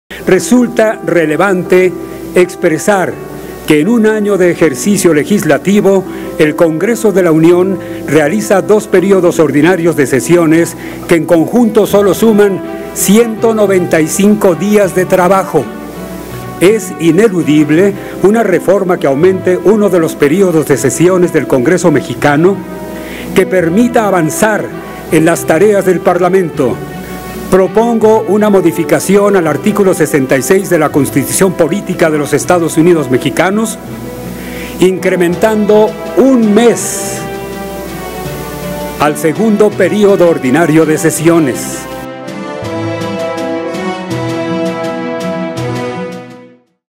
Que nuestro Congreso sesione tan poco tiempo es, sin duda, un freno
Que nuestro Congreso sesione tan poco tiempo es, sin duda, un freno 🛑 a las tareas parlamentarias, por esta razón las y los diputados de Morena proponemos incrementar un mes al segundo periodo ordinario de sesiones.📜 Mario Ismael Moreno Gil - Oficial